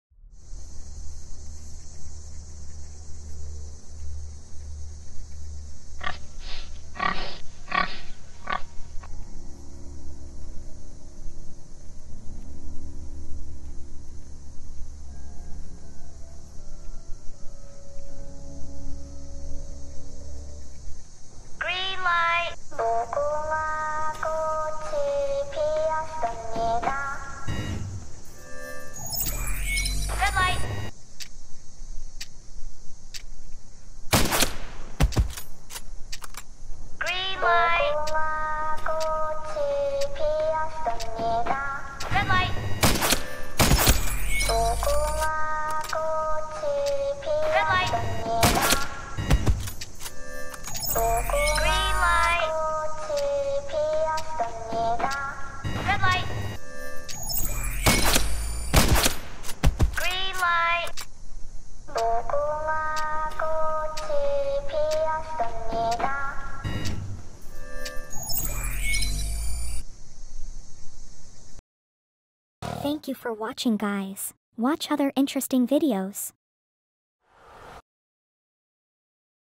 VR 360° Squid Game The Challenge - TEST YOUR STRENGTH!
Guys it's happened, the new season of Squid Game The Challenge is out. Let's reminisce and complete the challenge with the killer doll...